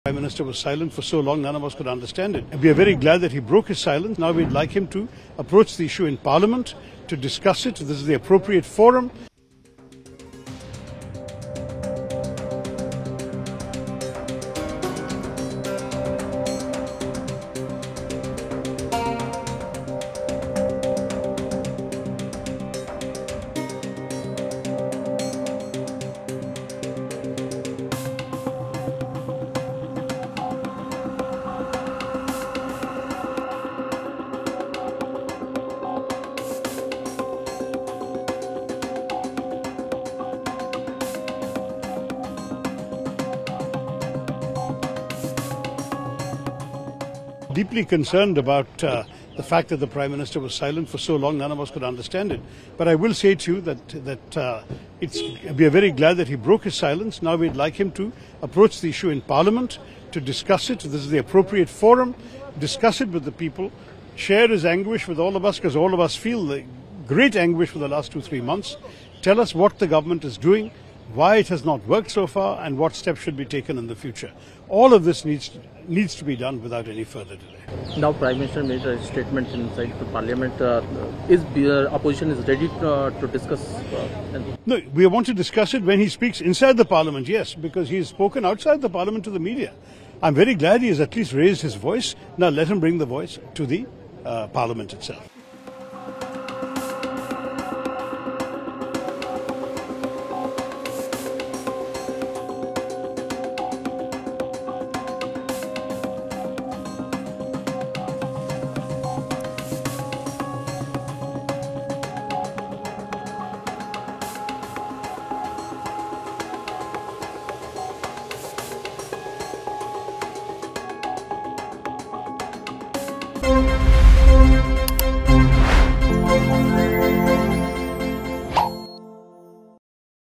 Shashi Tharoor's Sharp Comment On PM Modi's Address To Manipur Violence
Expressing relief that Prime Minister Narendra Modi has broken silence on a Manipur viral video of sexual assault, Congress leader Shashi Tharoor said that the former needs to raise his voice in the Parliament too. “The Prime Minister was silent for so long, we couldn’t understand it.